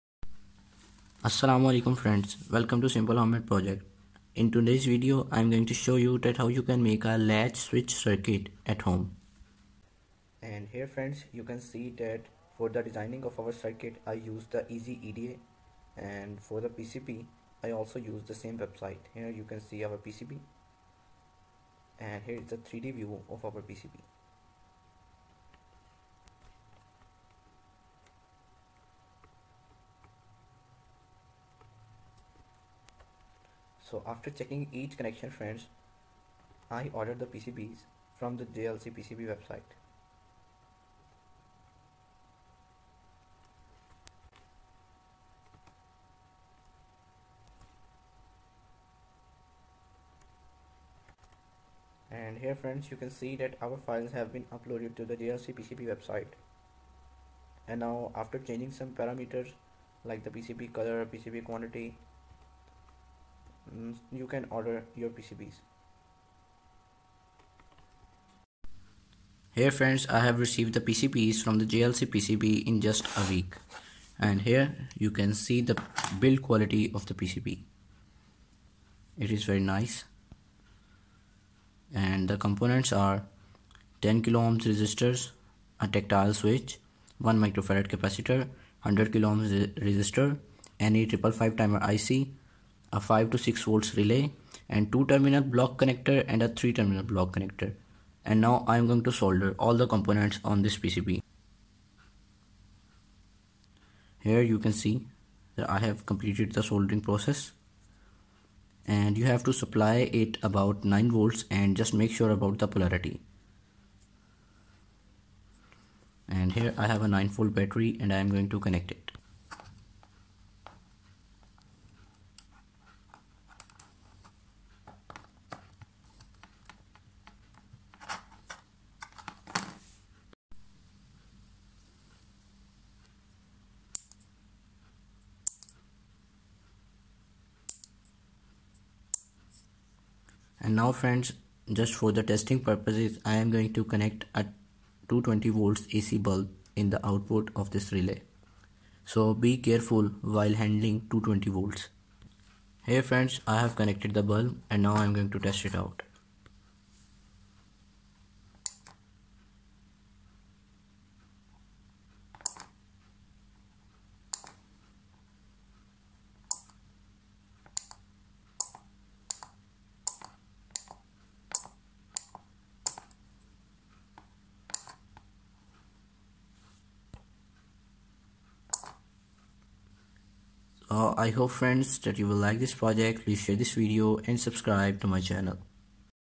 How to make a latch switch at home ?
JLCPCB link in new video will be:  $2 for 10 PCBs & 24 Hour Production:

Assalam O Alikum friend's , welcome to Simple Homemade Project's . In this video I made a latch switch circuit . Components required to make this project are given below :

1. PCB's
2. 5 volt's relay
3. IC 555 timer with socket
4. Tactile switch
5. 2 terminal block connector
6. 3 terminal block connector
7. 1 uf capacitor
8. 10 kilo OHM's resistors = 2
9. 100 kilo ohm resistor